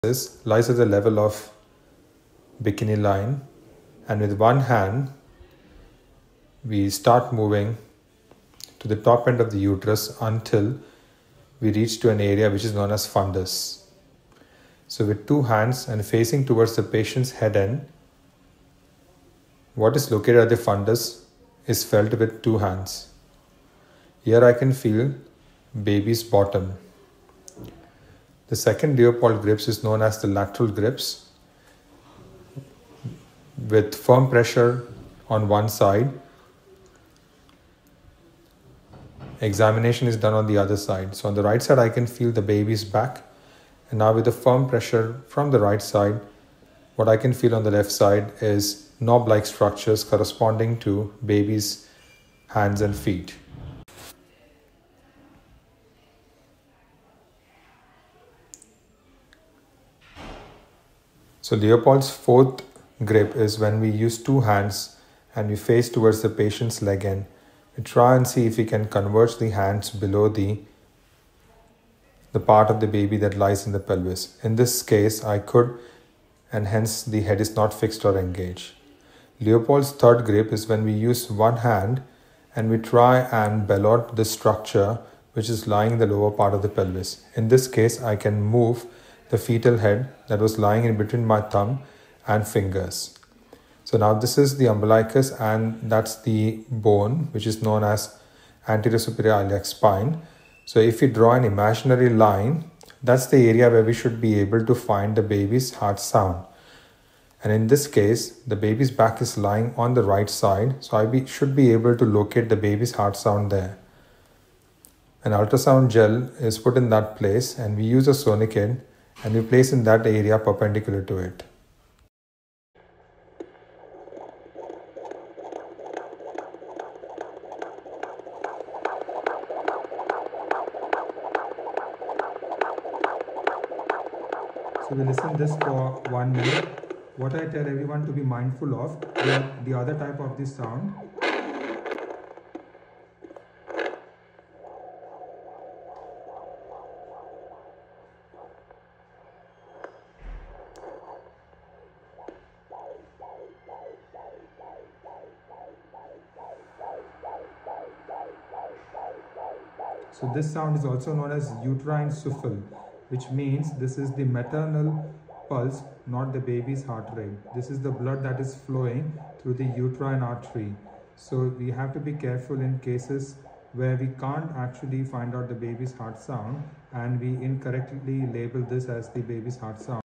Leopold's Maneuvers Explained: Find Fetal Position Fast!
Leopold's Maneuvers Explained: Find Fetal Position Fast! Dr. Kunal Rathod

Master the Leopold's Maneuvers with this essential clinical demonstration! Learn the four fundamental grips (Fundal, Lateral, Pawlik's, and Pelvic) used to accurately determine the fetal lie, presentation, and position in the pregnant uterus [00:09].

We also walk you through how to correctly locate the fetal heart sound [01:34] and, critically, how to differentiate it from the maternal uterine souffle [02:40], a common source of error. This video is a must-watch for medical students, nursing students, midwives, and anyone preparing for a clinical obstetrics exam!